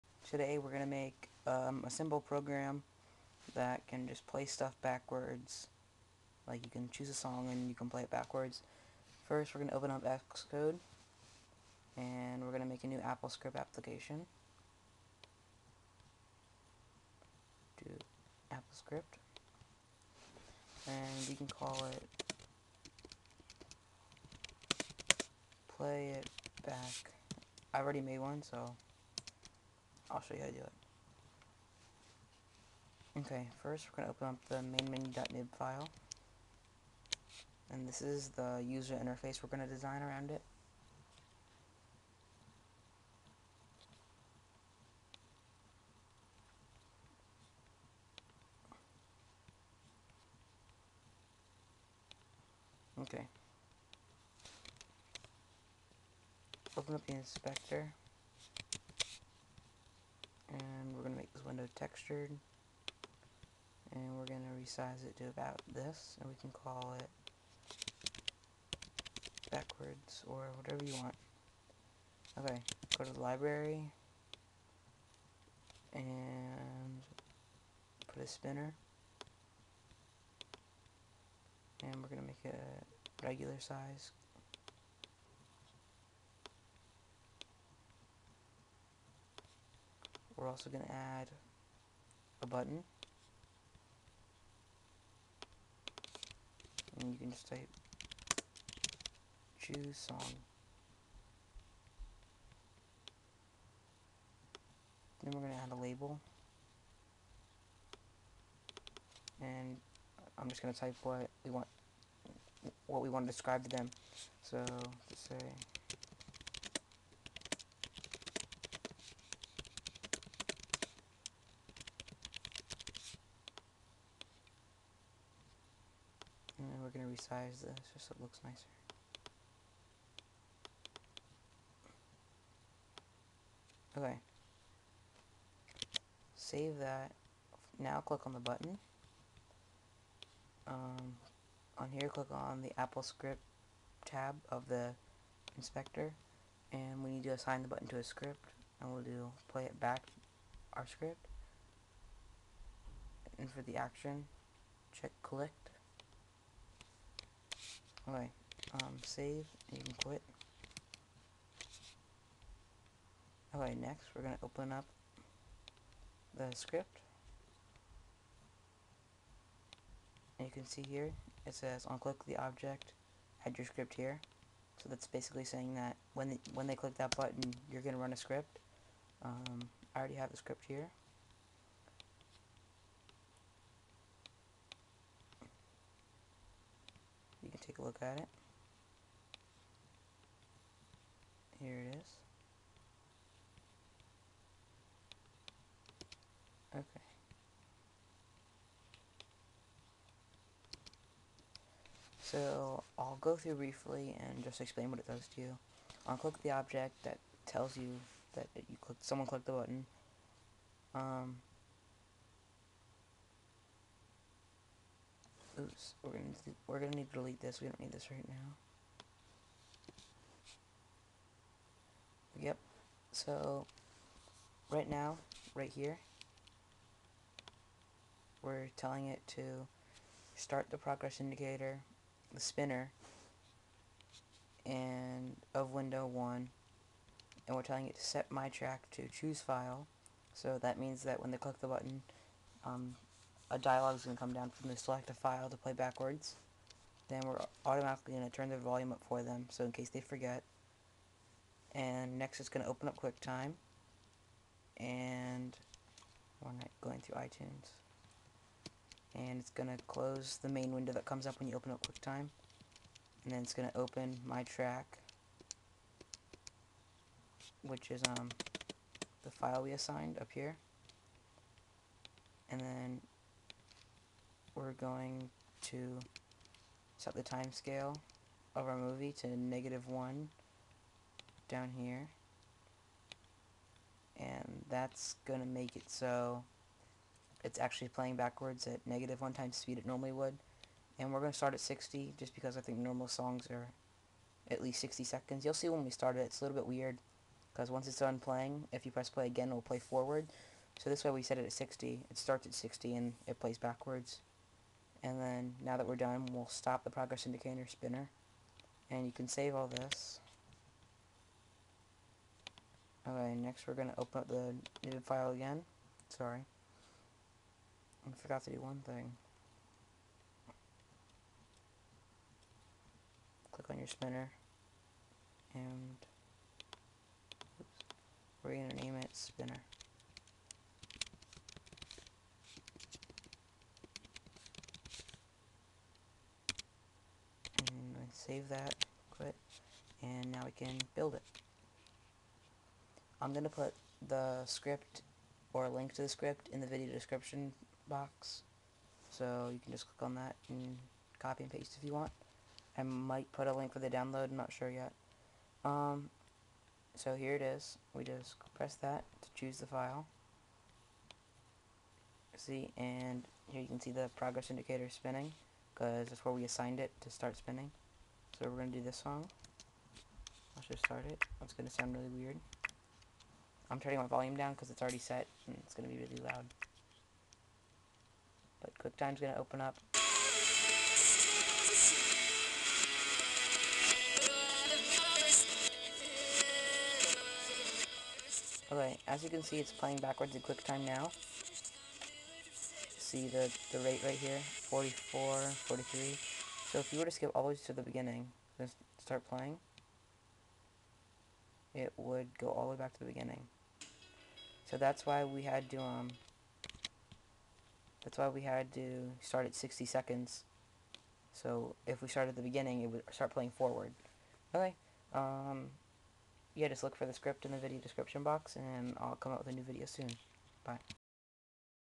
"Play it Backwards" Using Xcode and Applescript
on clicked theObject
 start progress indicator "Spinner" of window 1
 set my_track to choose file

 set volume 7
 --second, open QuickTime and start playing
 tell application "QuickTime Player"

  close every window
  open my_track
  set my_movie to first document
  set ts to time scale of my_movie
  set current time of my_movie to 60 * ts
  --start playing backwards
  set rate of my_movie to -1
 end tell
 stop progress indicator "Spinner" of window 1

end clicked

// Link Coming Soon, download all of my projects \\